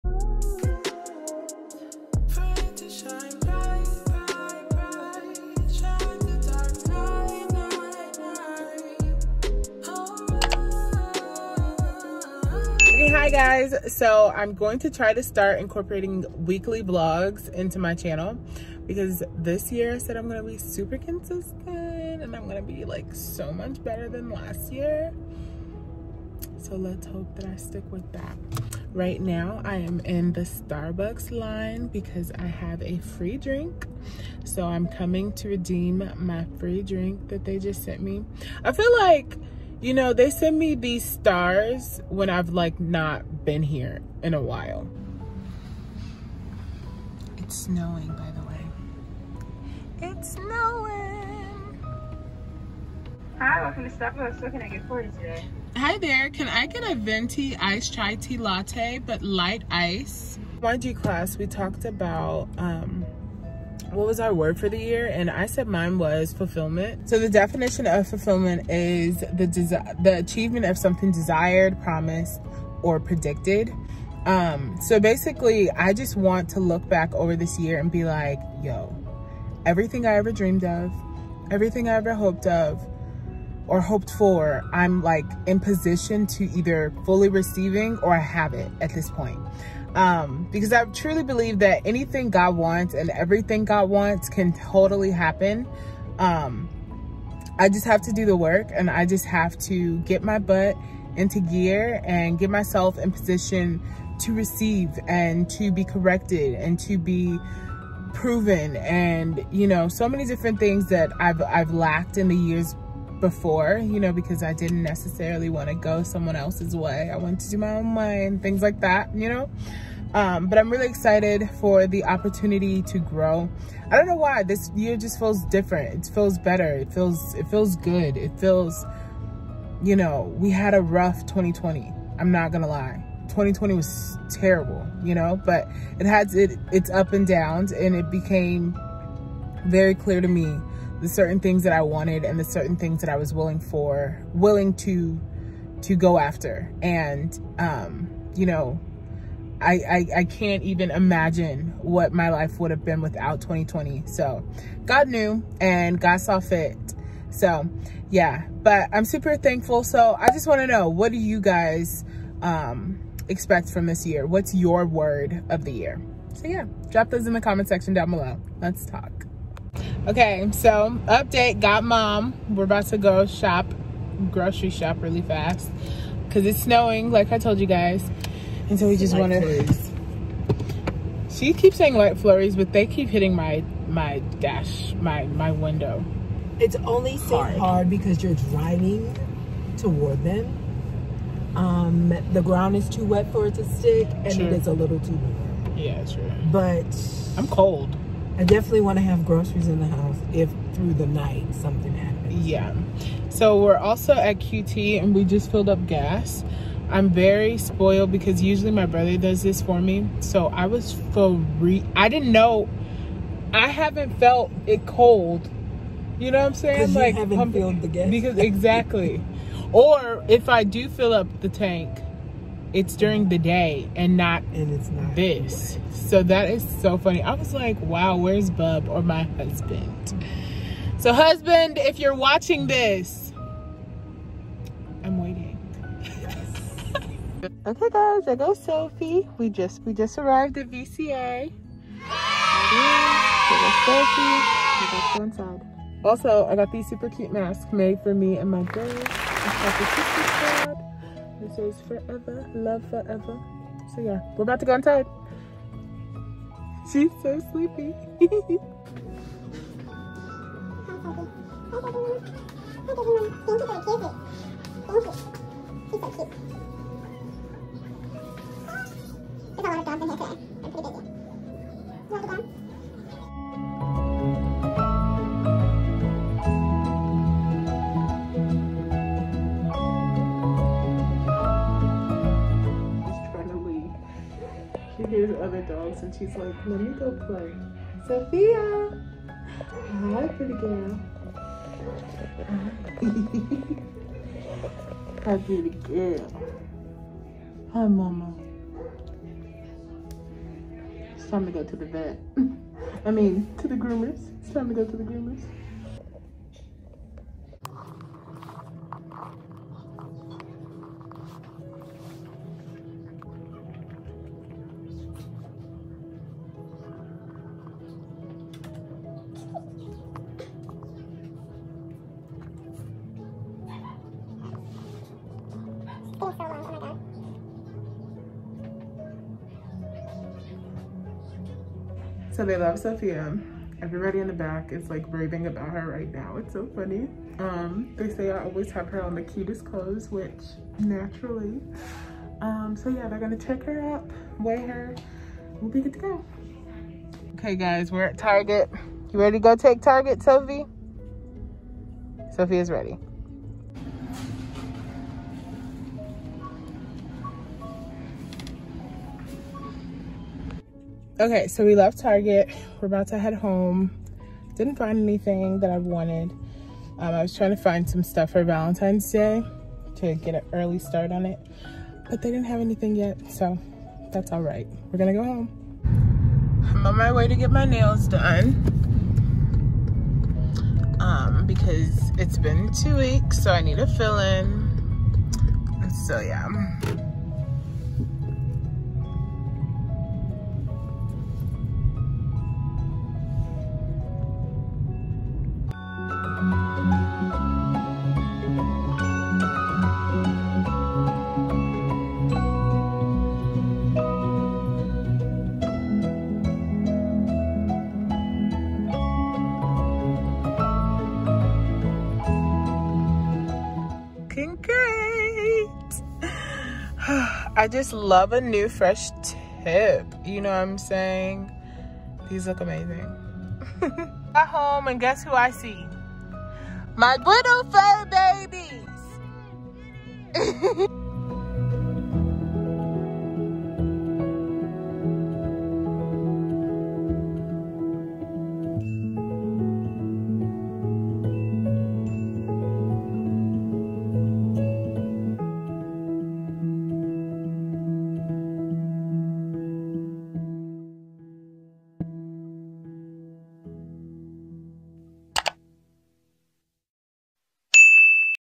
WKLY VLOG #1 | Sophia hangin' ❤️🥰✨
Hi guys!! I'm working on creating more content for you all because why not? :) Here's a random look at my life this past week! From doggy needs, target runs & self-care I just wanted to show you guys a bit into my world! I hope you enjoy it!!